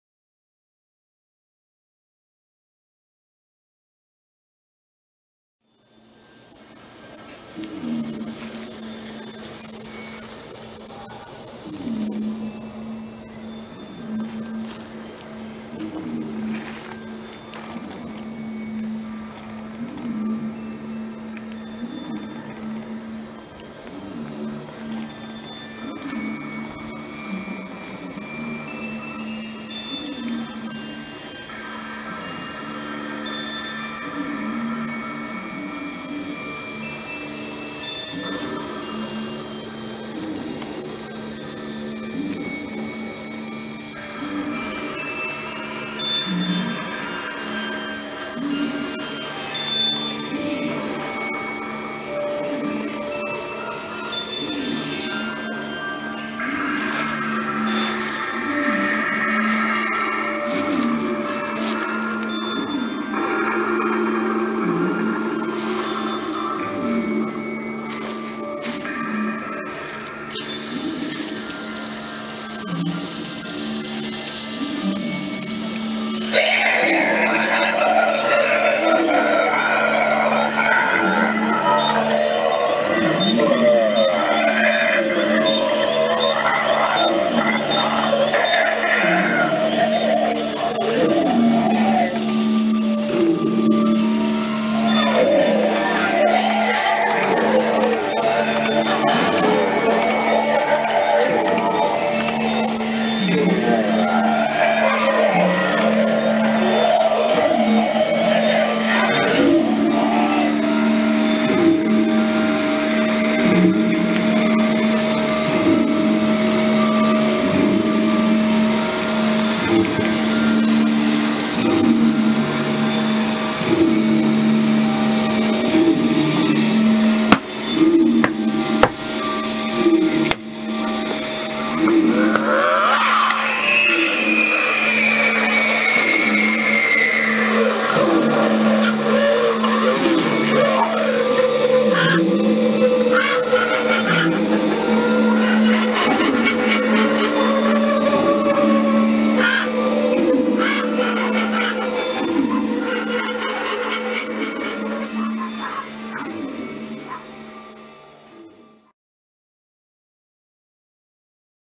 Halloween 2016 at Chateau Papillon
A video tour of the Halloween decorations we set up for 2016 at Chateau Papillon.